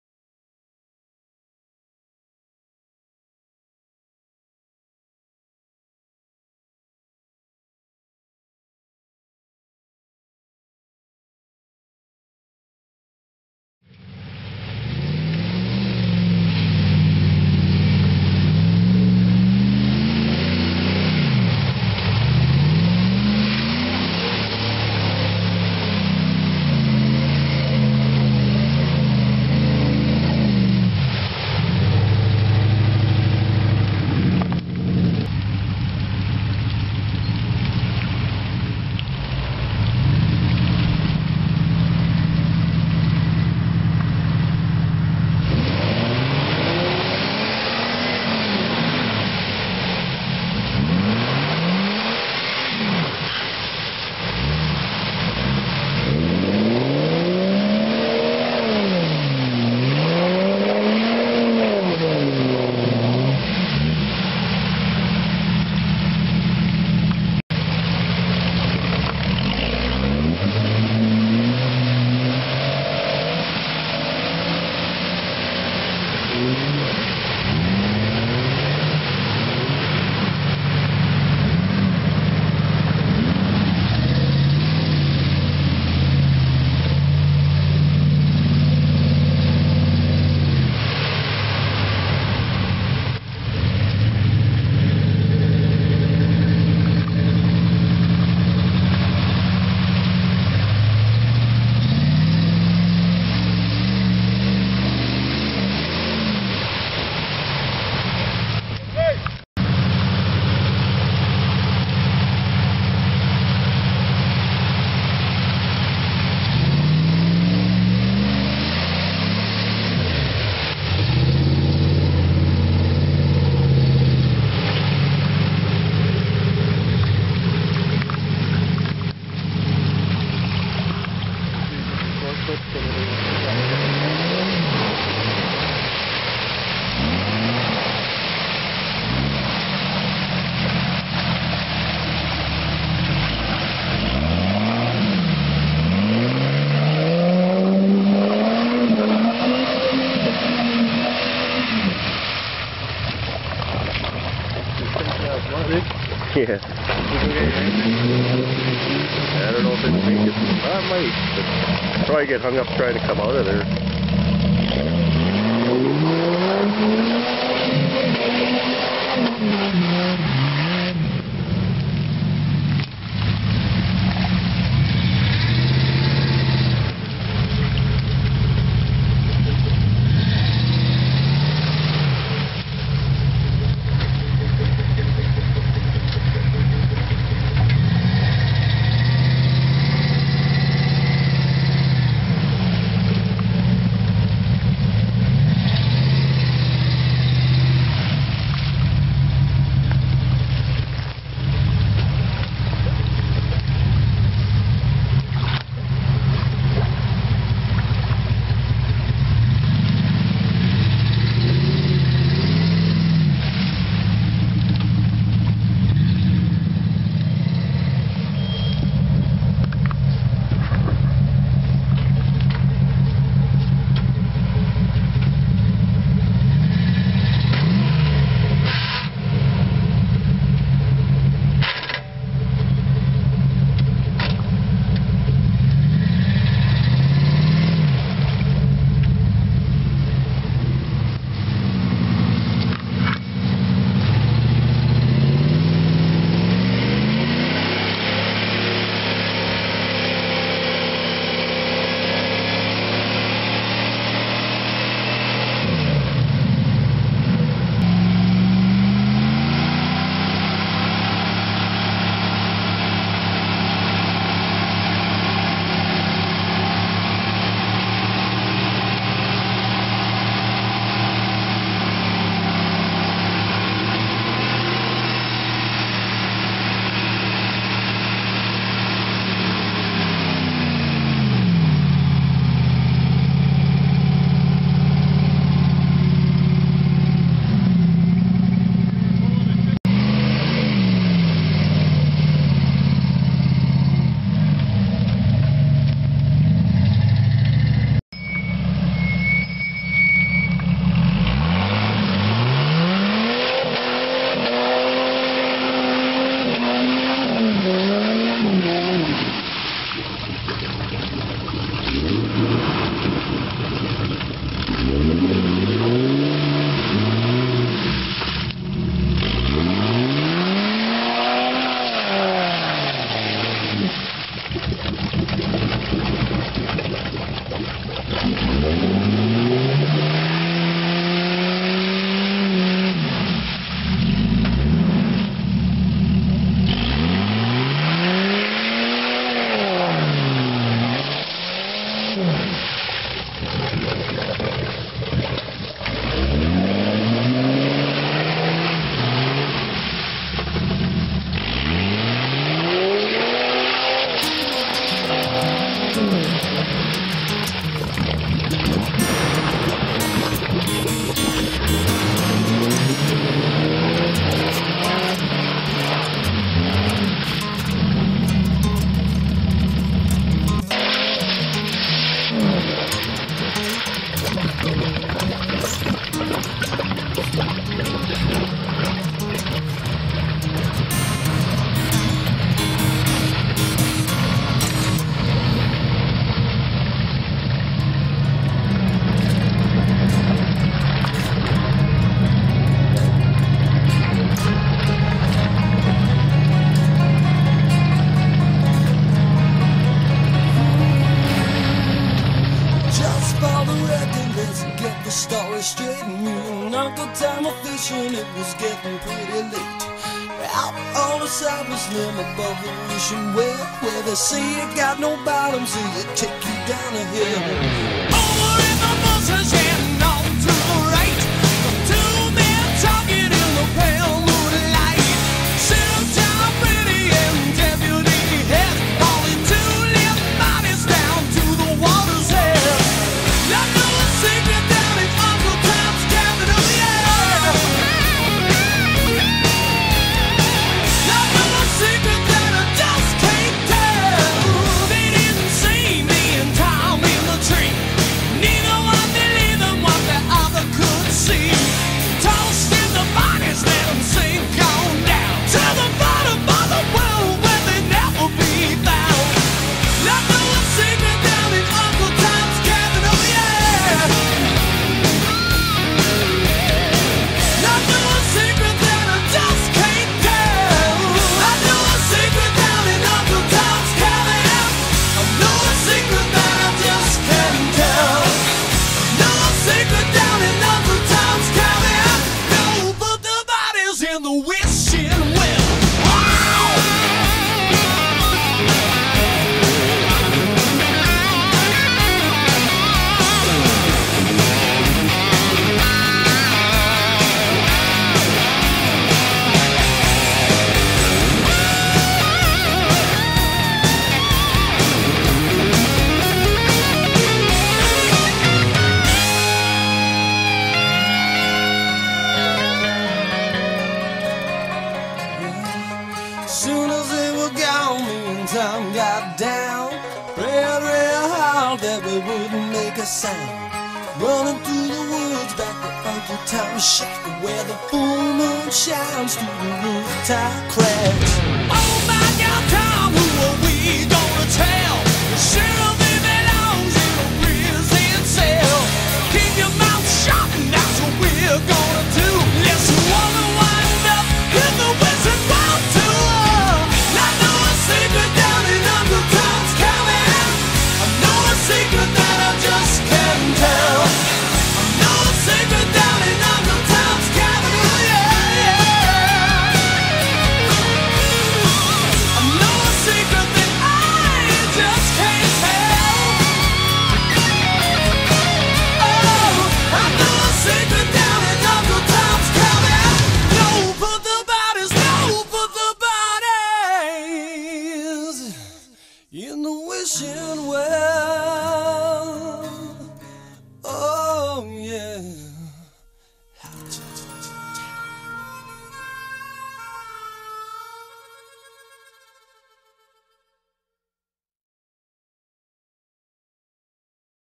4X4 Wikens lake HD
4X4 muddin with sum buds out by Wikens lake in da Dodge and Jeep